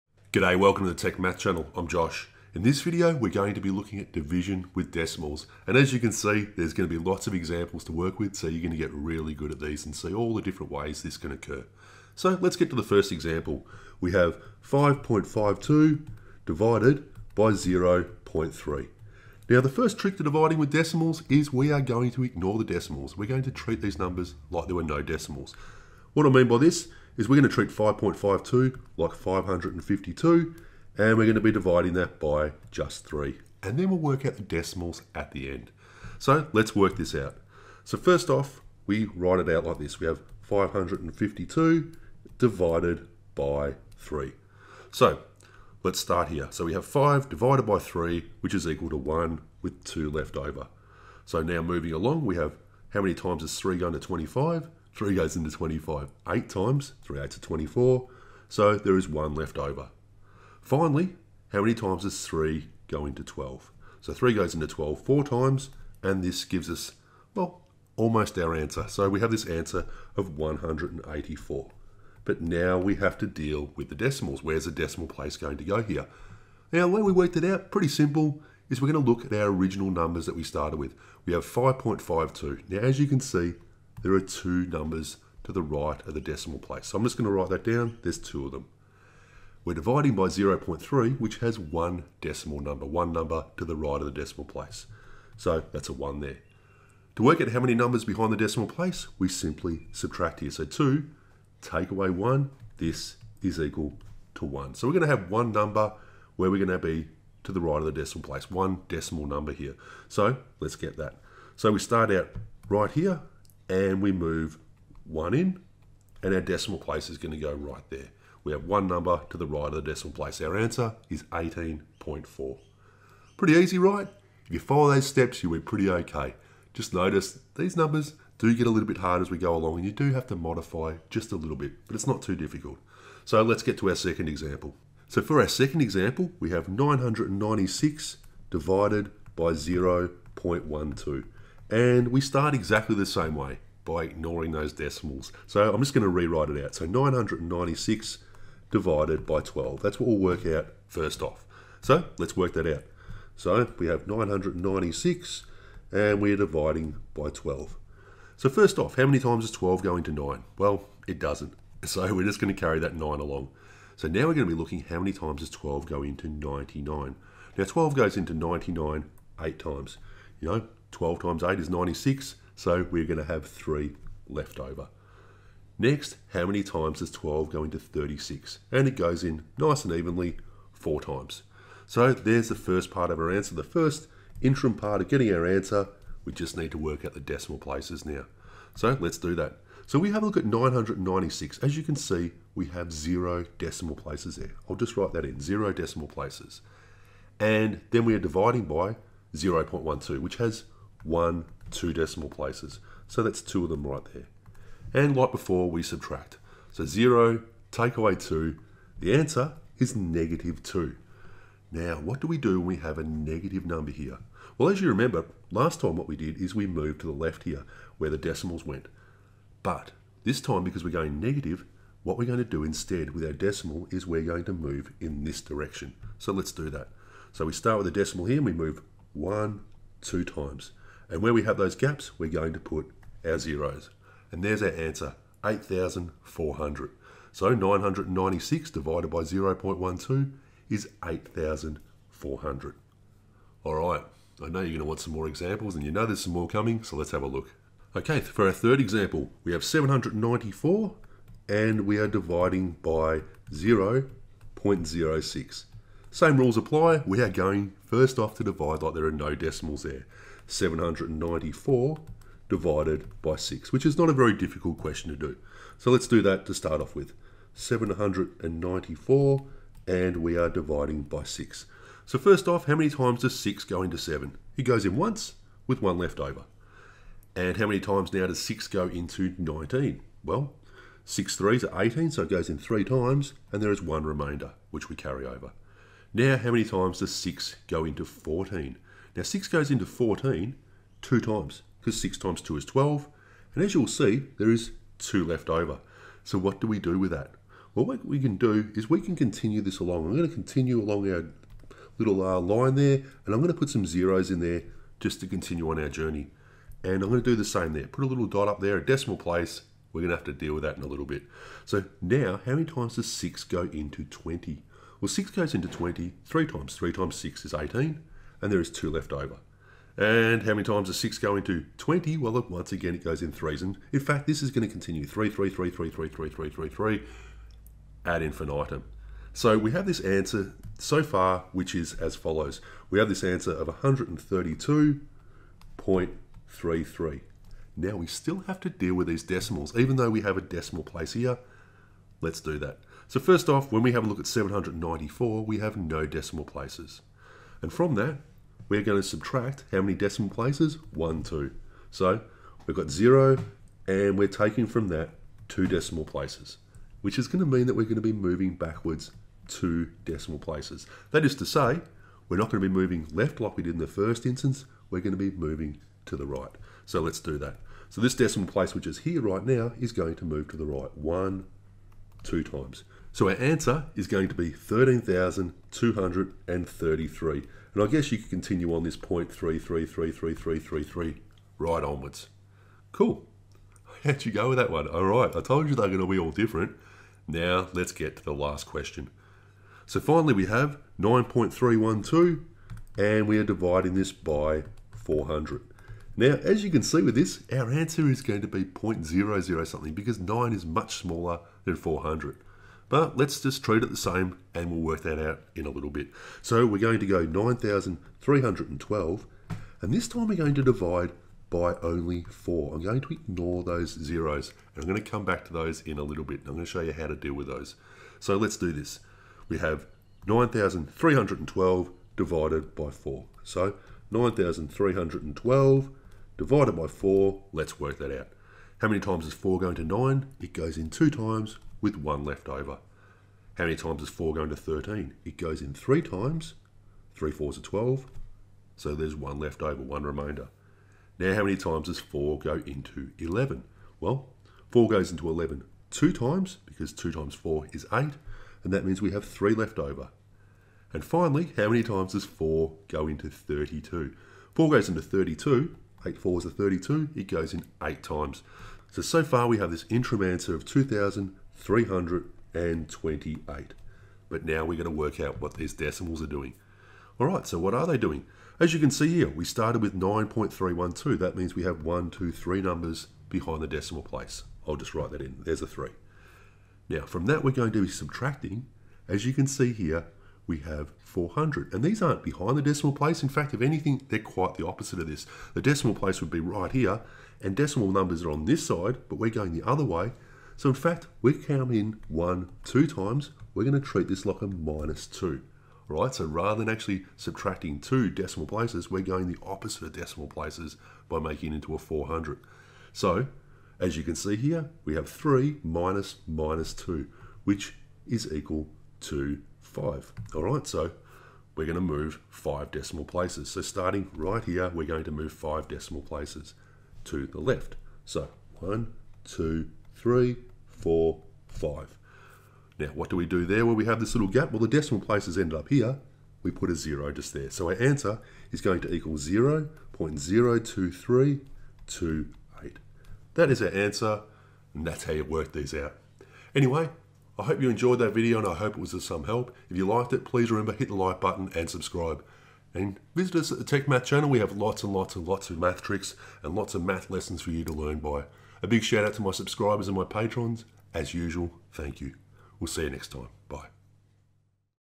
dividing decimals
How to divide decimals: first divide as if there is no decimal. Next, count the number of digits after the decimal in each number. Through a bit of subtraction you can work out where the decimal goes.